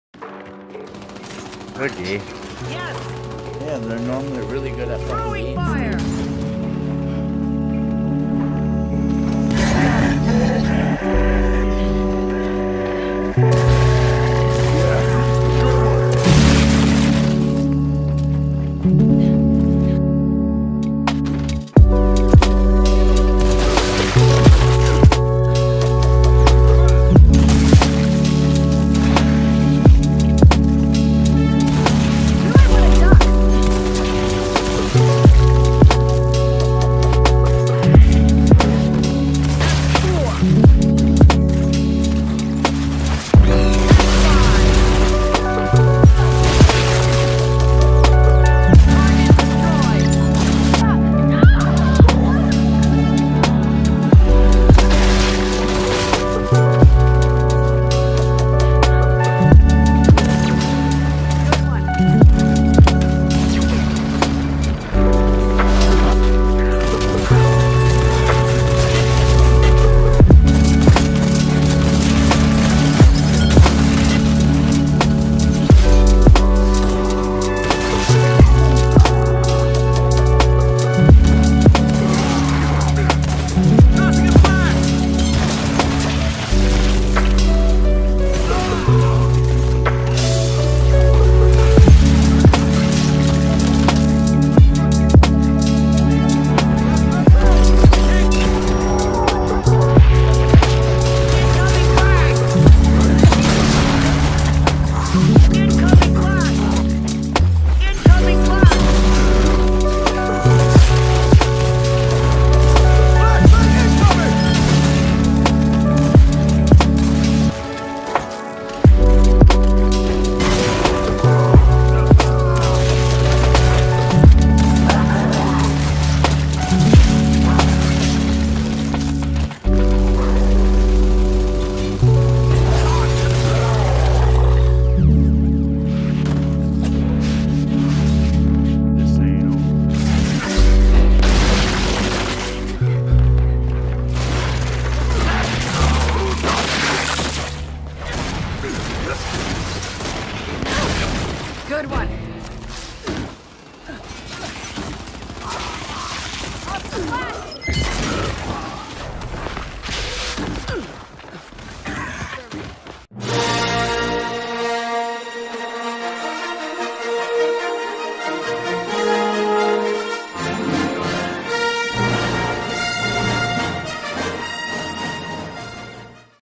Quads #1 Jedii Style (old Truly clips)
pc clean out, old truly quads that have been on my computer forever! LOL..  Thanks for checking out the video, Cheers!